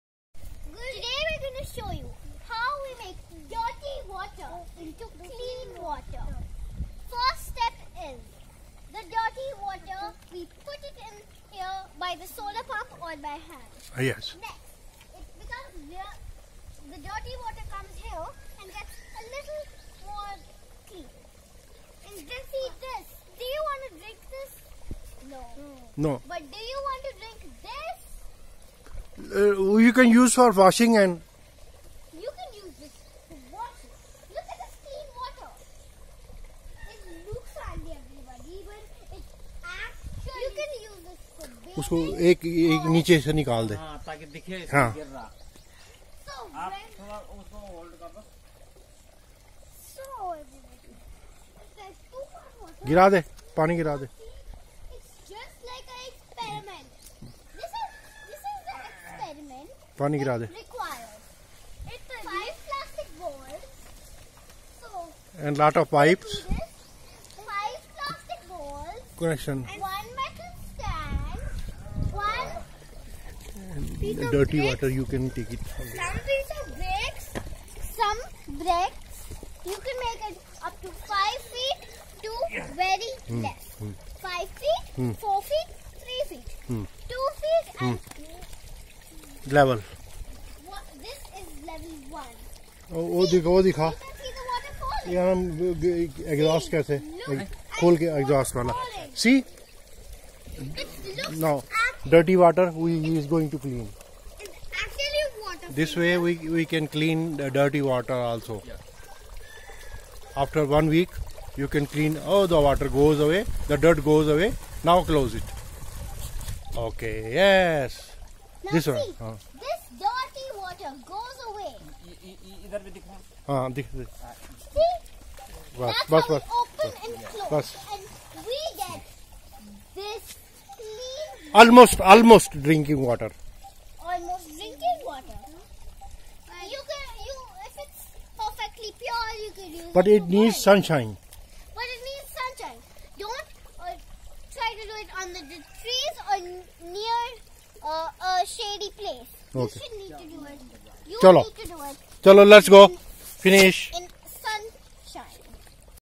OISCA BALJIT ORGANIC FILTER
Mud water converted into usable water. By sun light only. You need solar mini pump and granules sand only. Have a look